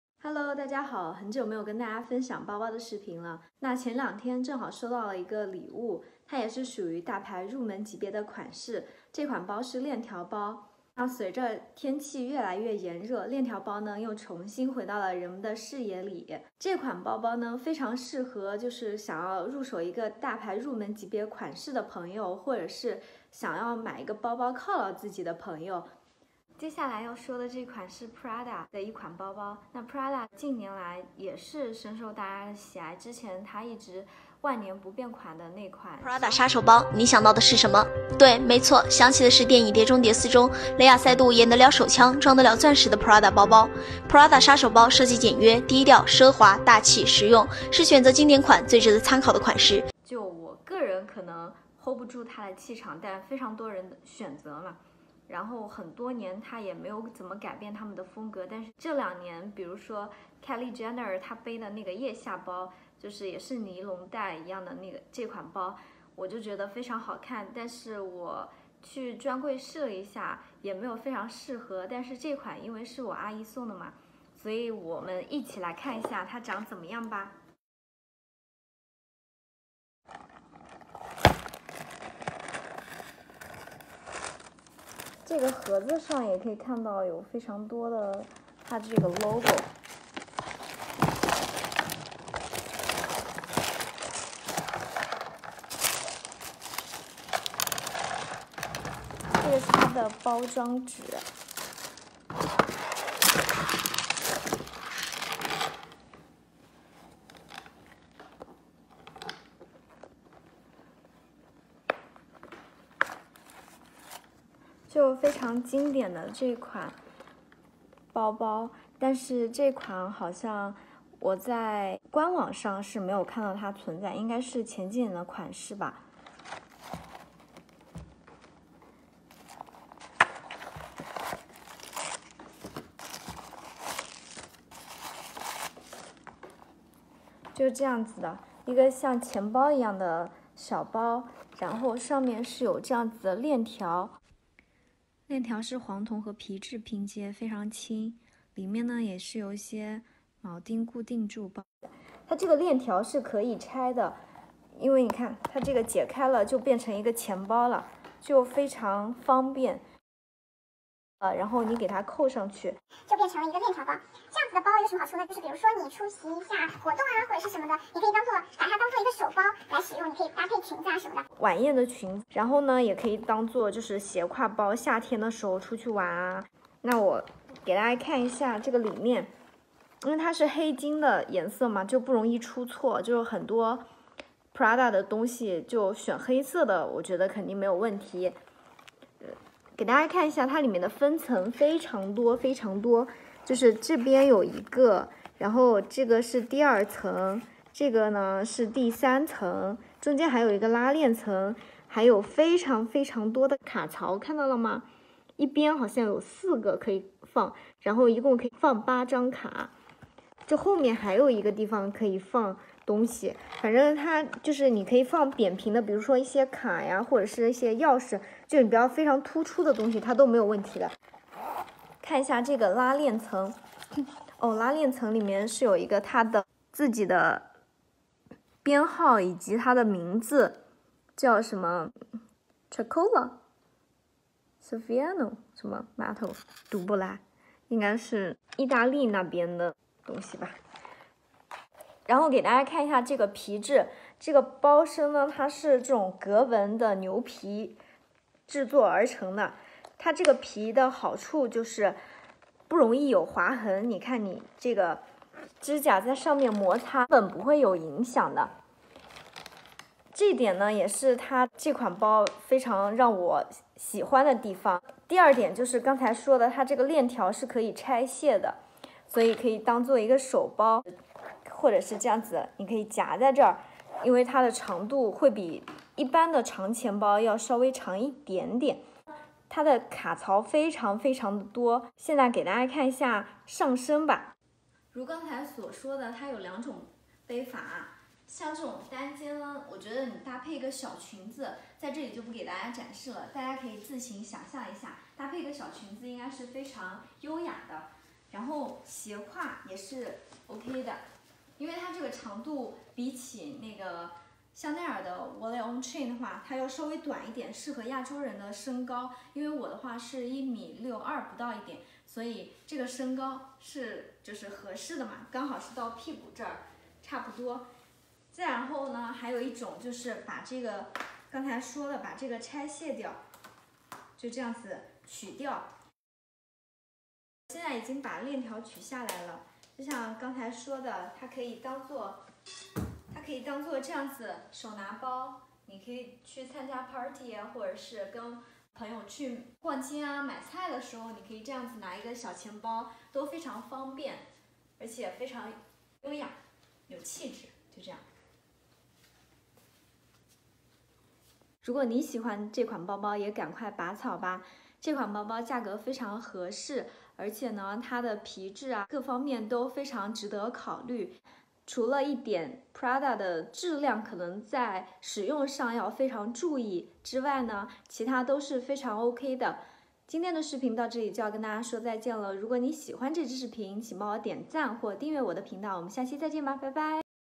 Prada | 预算$600 能买什么大牌包？｜和我一起开箱Prada包包｜unboxing prada
很长时间没有录包包视频，最近正好想做一期大牌入门级包包的视频合辑，这是第一期，包包特别适合夏天，大家快去视频做参考吧